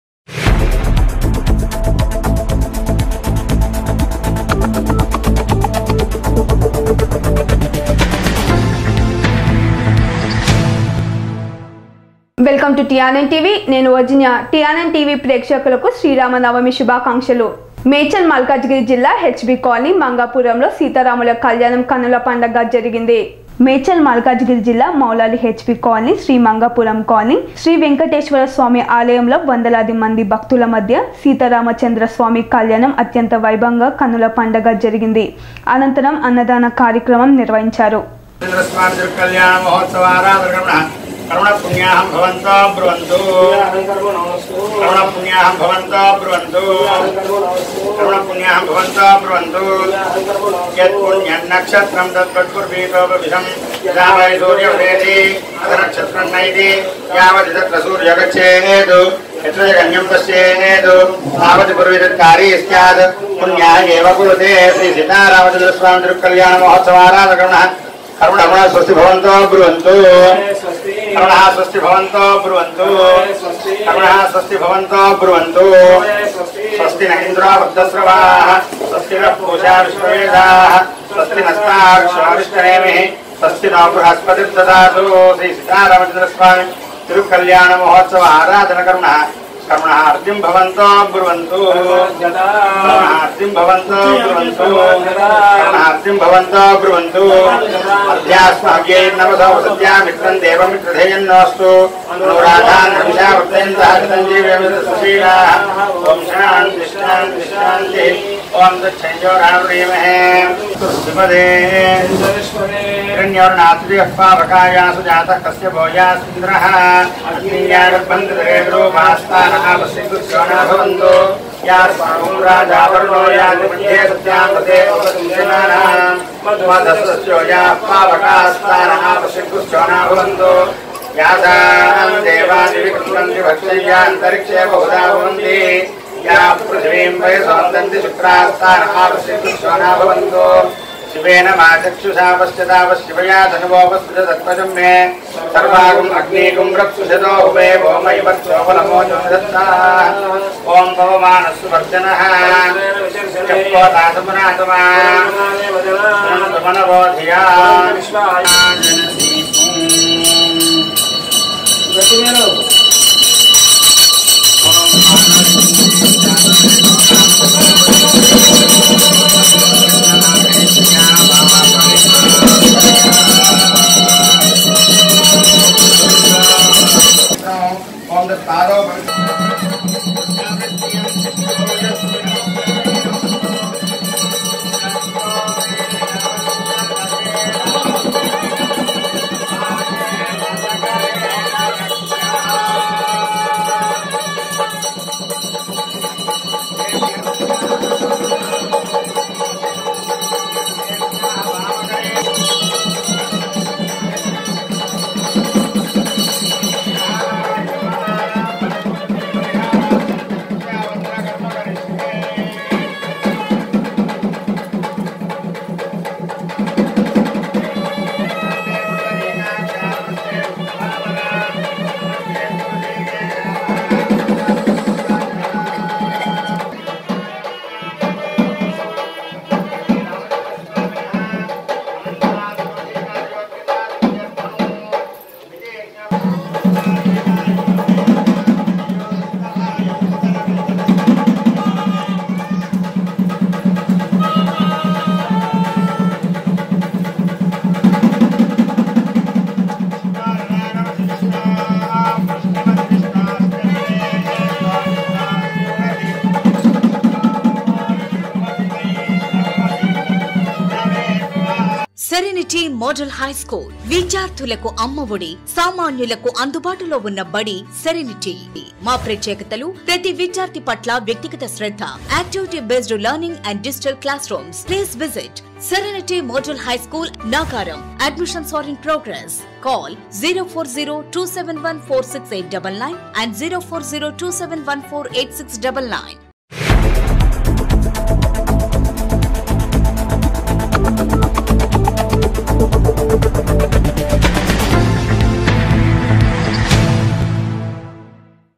మౌలాలి HB కాలనీ లో ఘనంగా శ్రీరామనవమి వేడుకలు||TR9 TV
మౌలాలి HB కాలనీ లో ఘనంగా శ్రీరామనవమి వేడుకలు